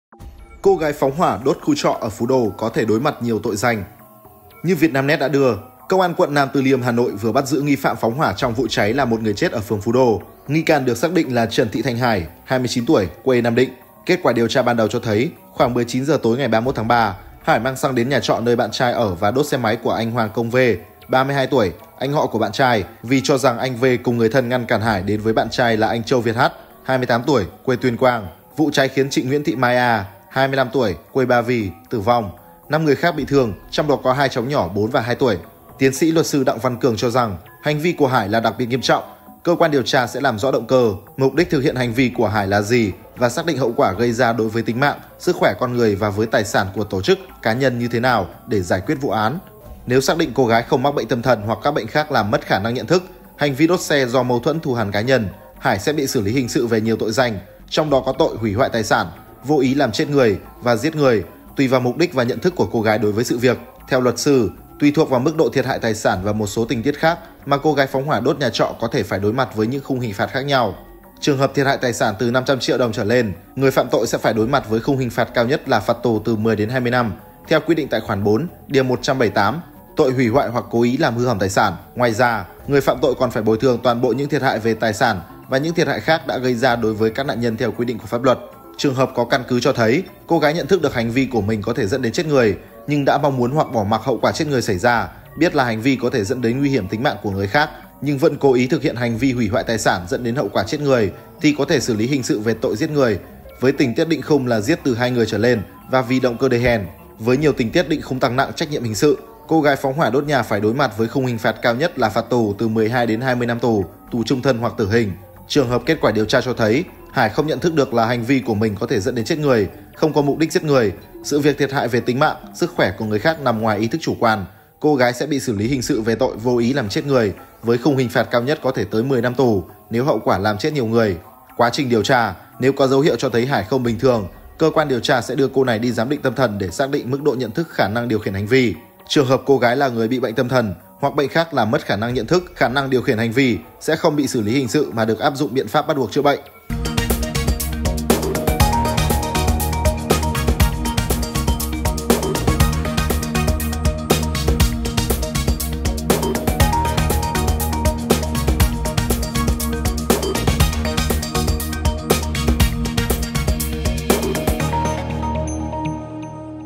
Cô gái phóng hỏa đốt khu trọ ở Phú Đô có thể đối mặt nhiều tội danh | Vietnamnet
Quá trình điều tra, nếu có dấu hiệu cho thấy Hải không bình thường, CQĐT sẽ đưa cô gái này đi giám định tâm thần để xác định mức độ nhận thức, khả năng điều khiển hành vi.

Như VietNamNet đã đưa, Công an quận Nam Từ Liêm (Hà Nội) vừa bắt giữ nghi phạm phóng hỏa trong vụ cháy làm 1 người chết ở phường Phú Đô.

Nghi can được xác định là Trần Thị Thanh Hải (29 tuổi, quê Nam Định).

Kết quả điều tra ban đầu cho thấy, khoảng 19h tối 31/3, Hải mang xăng đến nhà trọ nơi bạn trai ở và đốt xe máy của anh Hoàng Công V. (32 tuổi, anh họ của bạn trai), vì cho rằng anh V. cùng người thân ngăn cản Hải đến với bạn trai là anh Chẩu Việt H. (28 tuổi, quê Tuyên Quang).